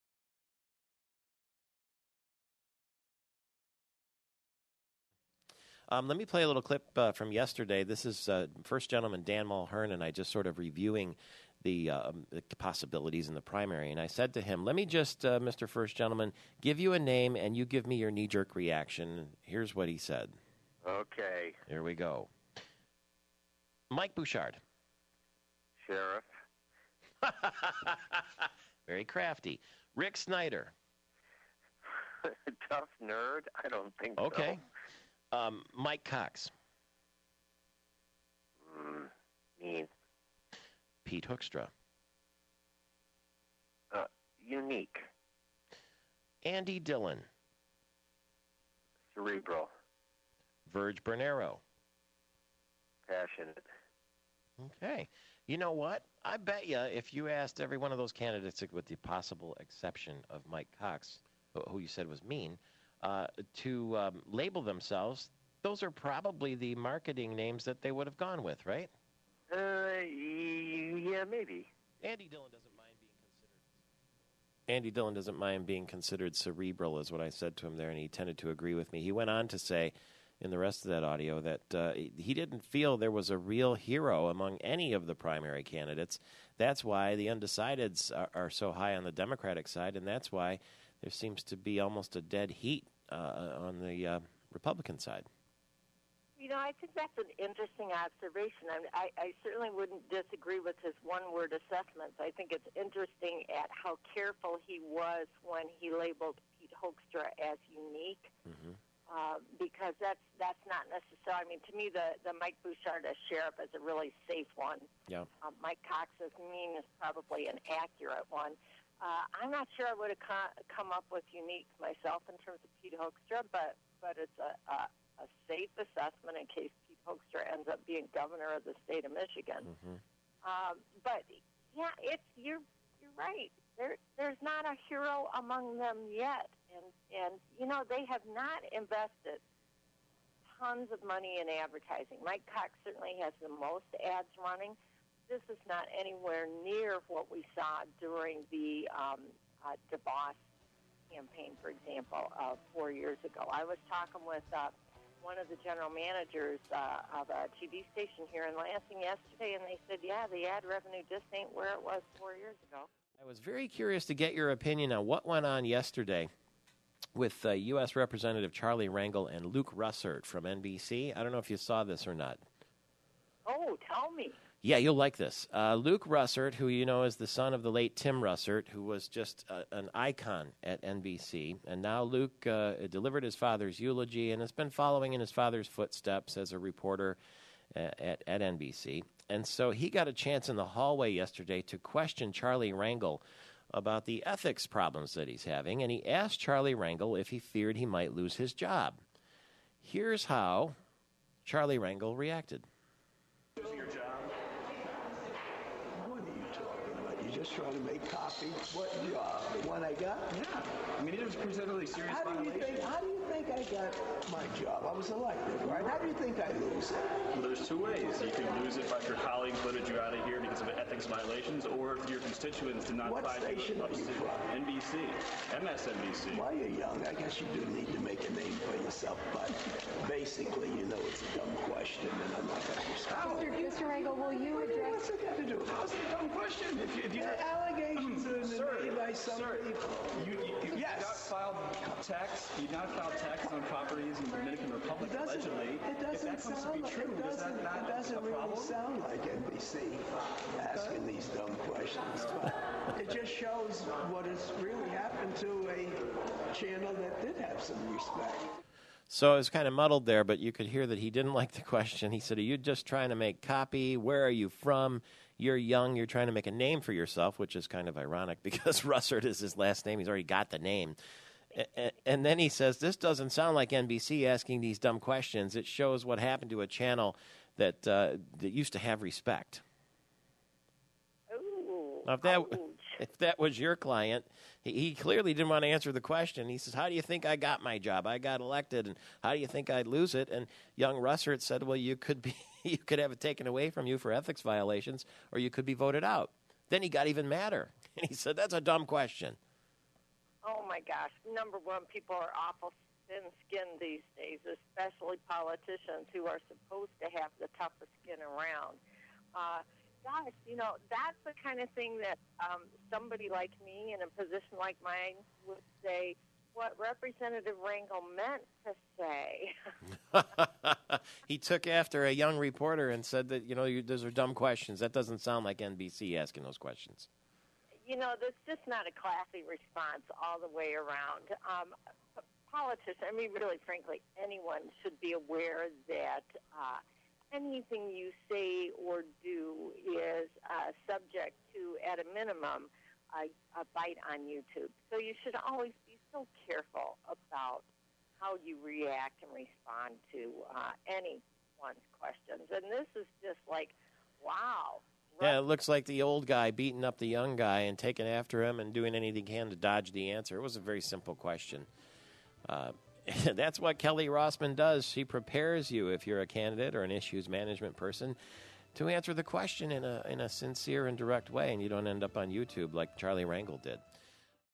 Kelly Rossman-McKinney on MPS
Kelly Rossman-McKinney discusses the Charlie Rangel/Luke Russert
questioning. Originally aired 7/23/10